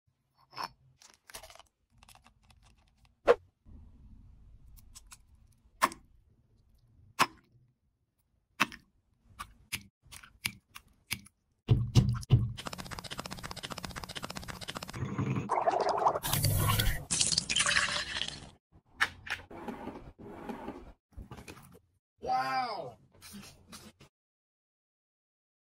Satisfying crushing, DIY ice cream.
DIY ice cream, use candy to make ice cream.